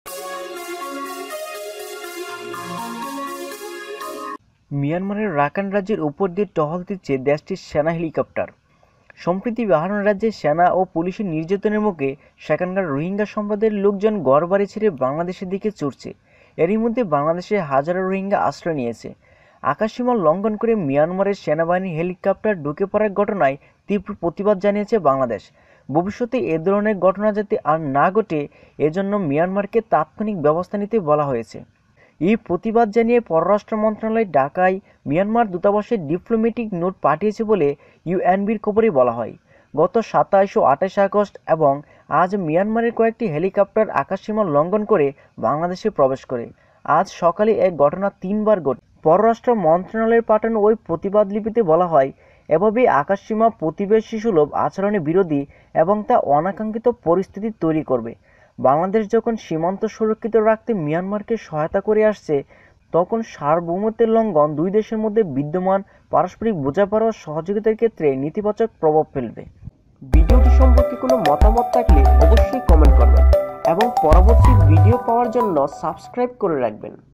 এই মাত্র পাওয়া খবরঃদেখুন মিয়ানমার বাংলাদেশকে আক্রমণ করার জন্য গোপনে কি ছক কষতেছে !
এই মাত্র পাওয়া খবরঃদেখুন মিয়ানমার বাংলাদেশকে আক্রমণ করার জন্য গোপনে কি ছক কষছে

আকাশসীমা লঙ্ঘন করে মিয়ানমারের সেনাবাহিনীর হেলিকপ্টার ঢুকে পড়ার ঘটনায় তীব্র প্রতিবাদ জানিয়েছে বাংলাদেশ। ভবিষ্যতে এ ধরনের ঘটনা যাতে আর না ঘটে, এ জন্য মিয়ানমারকে তাৎক্ষণিক ব্যবস্থা নিতে বলা হয়েছে।

আজ শুক্রবার এই প্রতিবাদ জানিয়ে পররাষ্ট্র মন্ত্রণালয় ঢাকায় মিয়ানমার দূতাবাসে ডিপ্লোমেটিক নোট পাঠিয়েছে বলে ইউএনবির খবরে বলা হয়।

গত ২৭ ও ২৮ আগস্ট এবং আজ মিয়ানমারের কয়েকটি হেলিকপ্টার আকাশসীমা লঙ্ঘন করে বাংলাদেশে প্রবেশ করে। আজ সকালেই এ ঘটনা তিনবার ঘটে।

পররাষ্ট্র মন্ত্রণালয়ের পাঠানো ওই প্রতিবাদলিপিতে বলা হয়, এভাবে আকাশসীমা প্রতিবেশীসুলভ আচরণের বিরোধী এবং তা অনাকাঙ্ক্ষিত পরিস্থিতি তৈরি করবে।